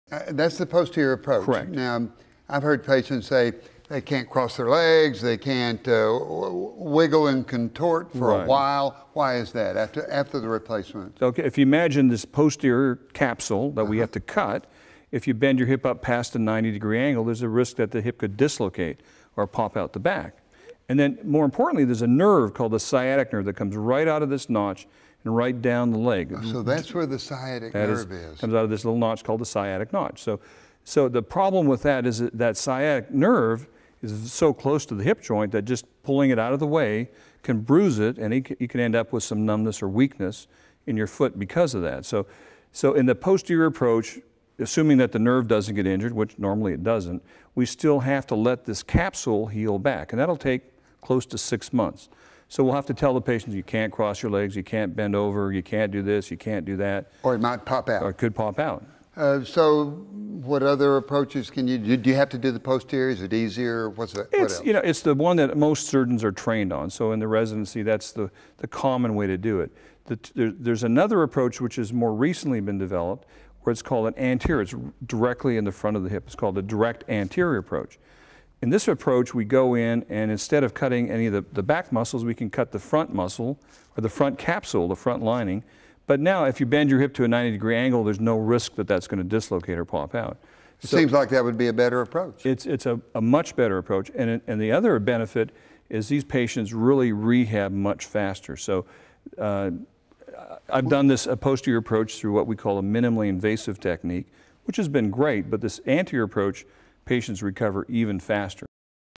Risks of Minimally Invasive Hip Replacement Surgery
Orthopedic surgeon Dr. Joseph Chenger explains common risks of hip replacement surgery (posterior approach) and touches on why the anterior approach is more effective.